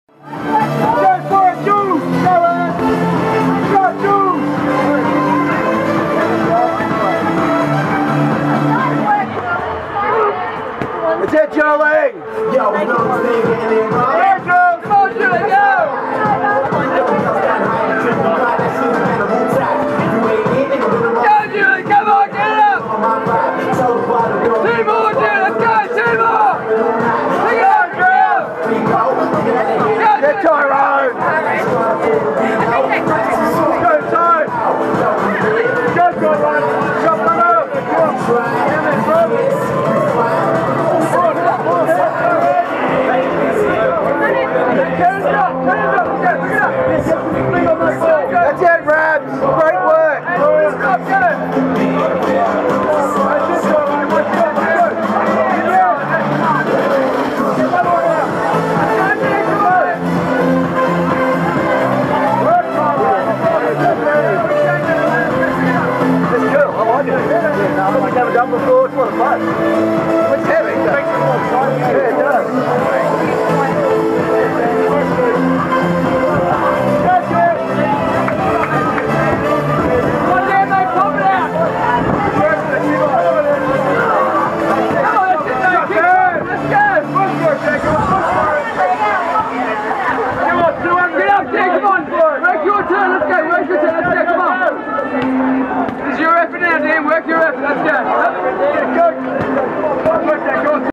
CrossFit BOSS Men's Team 1 55kg Deadball Ground to Over Shoulder at WODStock Finals 21-1-13
CrossFit BOSS's Mogsy Gav and Chriso
55kg Deadball Ground to Over Shoulder
Crabwalk